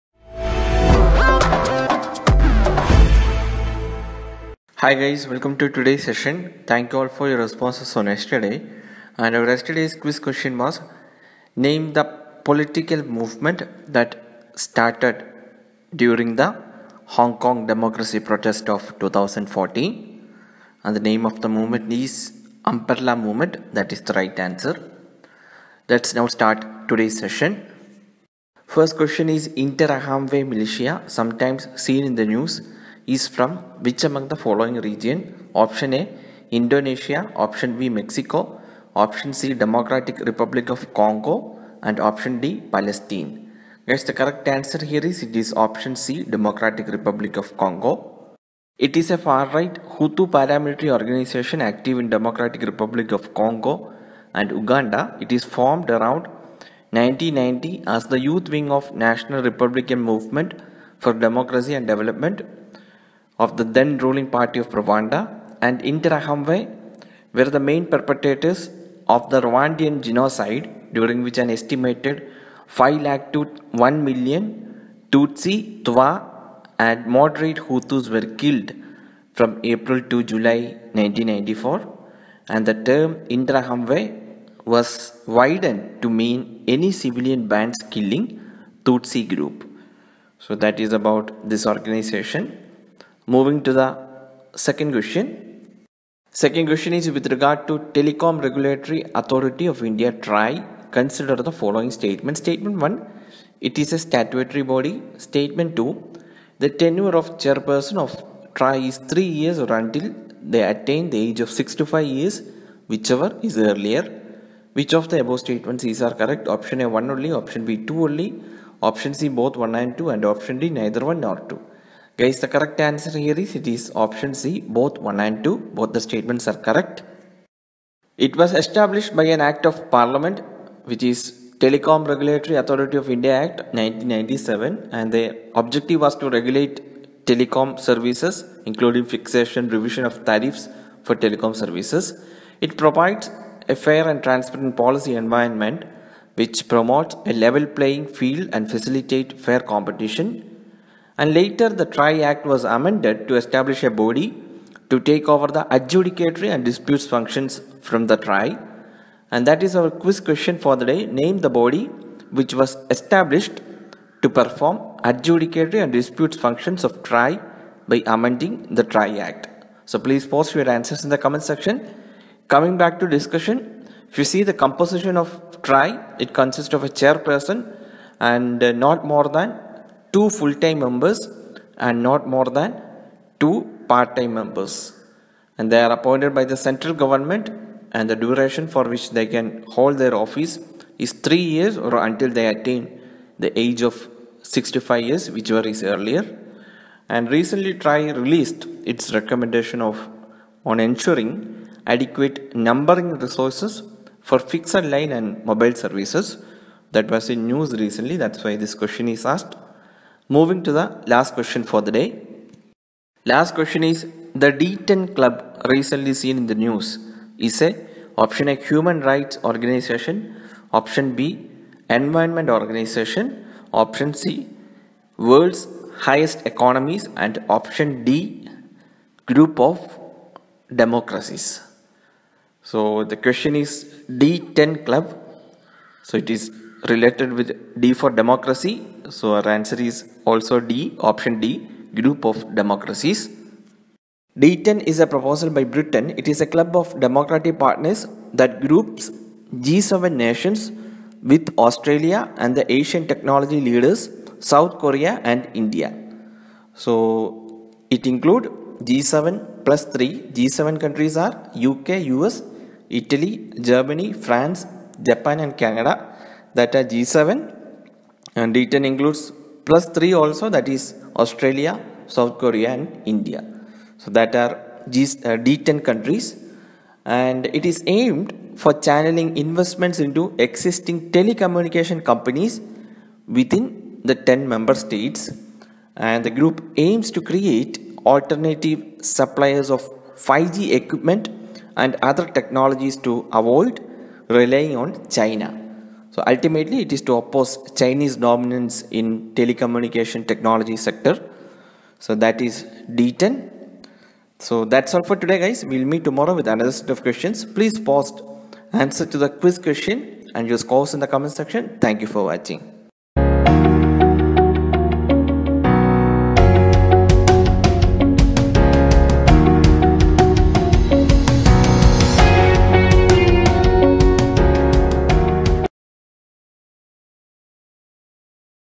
Most Probable Questions for IAS Prelims - Qns for 27-08-2020
FREE - ClearIAS Most Probable Prelims Questions (PDF sets): Exclusive for paid subscribers.

Qn.1) Interahamwe militia, sometimes seen in the news is from which among the following region?

(a) Indonesia
(b) Mexico
(d) Palestine

Learning Zone:
It is a far-right Hutu paramilitary organization active in the Democratic Republic of the Congo and Uganda.
Formed around 1990 as the youth wing of the National Republican Movement for Democracy and Development (MRND according to its French name), the then-ruling party of Rwanda.

Why is this question important? The Interahamwe were the main perpetrators of the Rwandan genocide, during which an estimated 500,000 to 1,000,000 Tutsi, Twa, and moderate Hutus were killed from April to July 1994, and the term “Interahamwe” was widened to mean any civilian bands killing Tutsi.

Qn.2) With regard to Telecom Regulatory Authority of India (TRAI), consider the following statements

1. It is a statutory body.
2. The tenure of Chairperson of TRAI is three years or until they attain the age of 65 years, whichever is earlier.

Learning Zone: It is a statutory body set up under section 3 of the Telecom Regulatory Authority of India Act, 1997.

It is the regulator of the telecommunications sector in India.

The TRAI Act was amended by an ordinance, effective from 24 January 2000, establishing a Telecom Disputes Settlement and Appellate Tribunal (TDSAT) to take over the adjudicatory and disputes functions from TRAI.

It consists of a Chairperson and not more than two full-time members and not more than two part-time members.
They are appointed by the Central Government and the duration for which they can hold their office is three years or until they attain the age of 65 years, whichever is earlier.

Why is this question important?  The Telecom Regulatory Authority of India (TRAI) has released its recommendations on “ensuring adequate numbering resources for fixed-line and mobile services”.

Qn.3) The D10 Club recently seen in news is a

(a) Human Rights Organisation
(b) Environment Organisation
(d) Group of democracies

Ans: (d) Group of democracies

Learning Zone: Britain is proposing a ‘D10’ club of democratic partners that groups the G7 nations with Australia and the Asian technology leaders South Korea and India.
It would include G7 countries – UK, US, Italy, Germany, France, Japan and Canada – plus Australia, South Korea and India.
It is aimed for channelling investments into existing telecommunication companies within the 10 member states.
The group aim to create alternative suppliers of 5G equipment and other technologies to avoid relying on China.

Why is this question important? It is a recent proposal